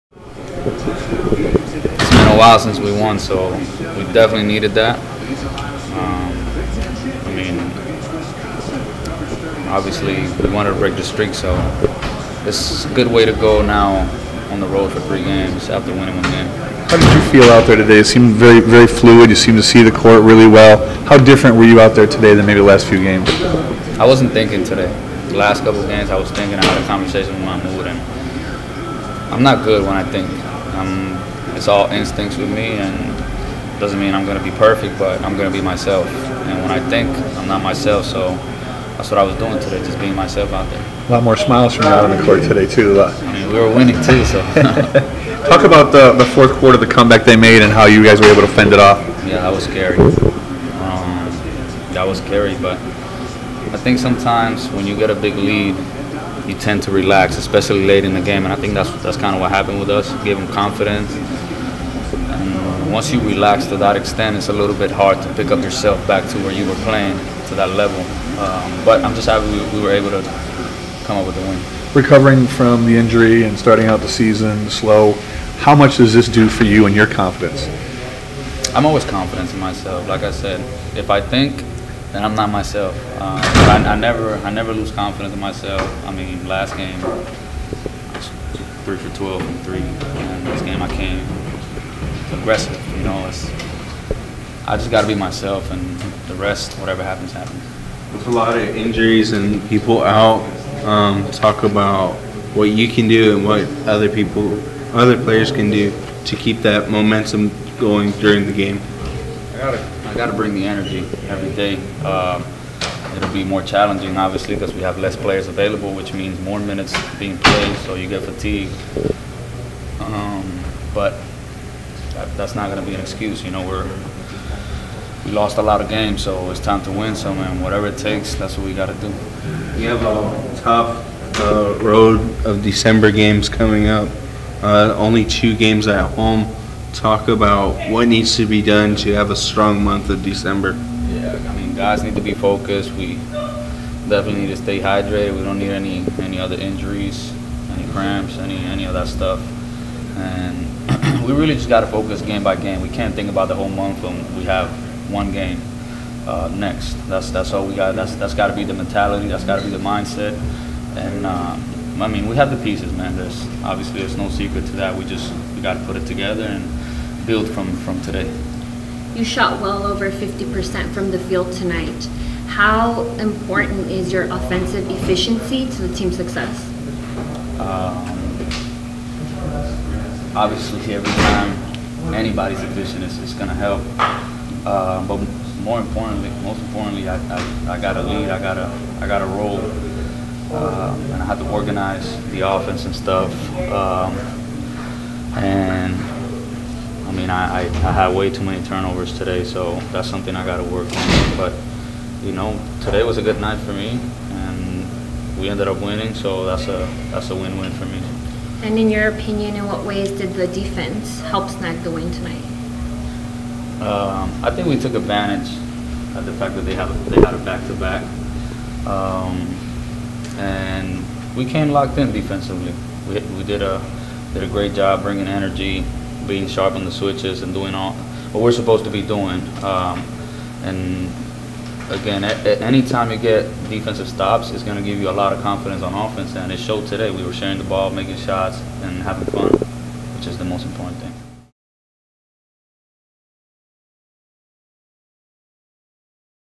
RGV Vipers guard Angel Rodriguez press conference vs SBL 113019
The RGV Vipers hosted the South Bay Lakers and guard Angel Rodriguez talks to the media after the game.